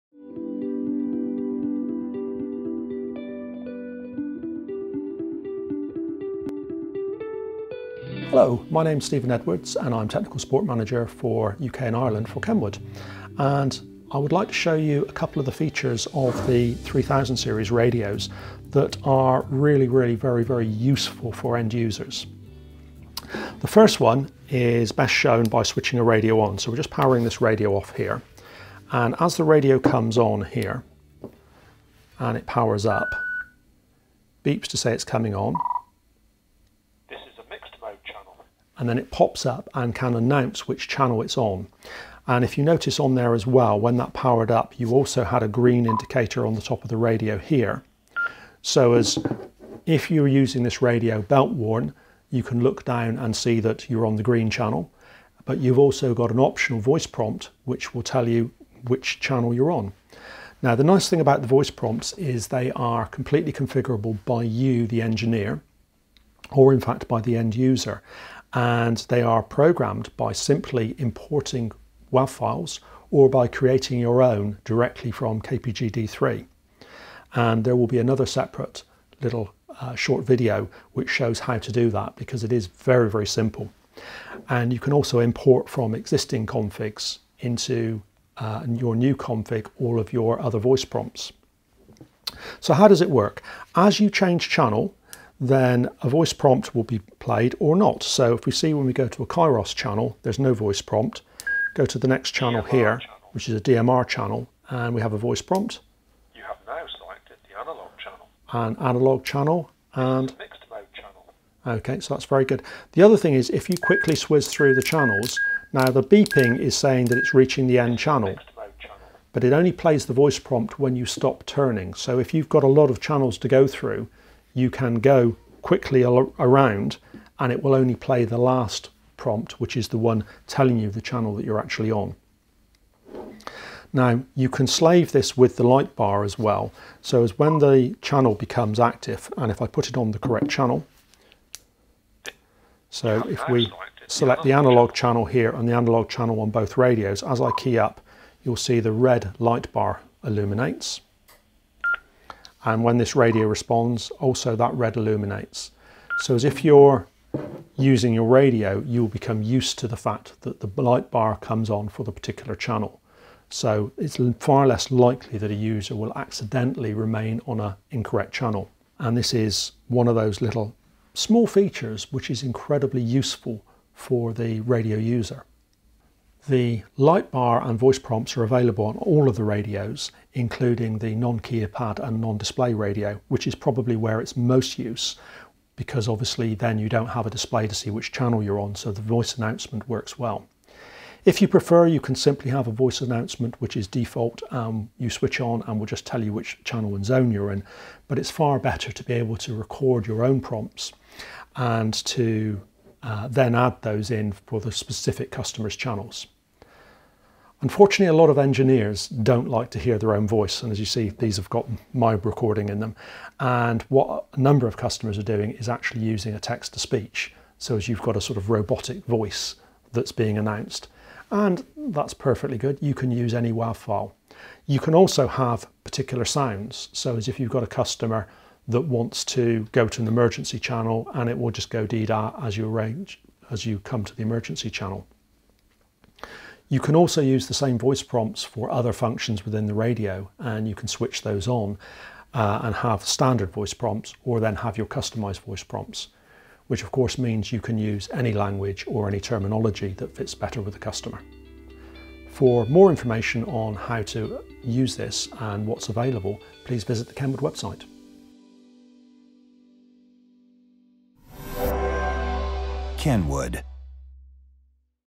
NX-3000 Series lightbar and custom voice prompts | Kenwood Comms
The lightbar and custom voice prompts are shown in operation on the NX-3000 series heandheld walkie talkie radio, these features are a big advantage for covert or belt warn radios helping to ensure the user is always on the correct channel.